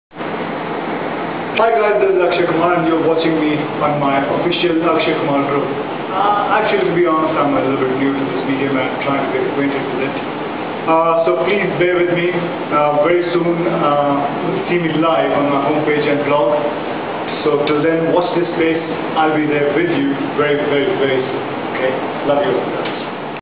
Akshay Kumar's first video blog
Check out Akshay Kumar's first video blog, shot back in the day in 2009.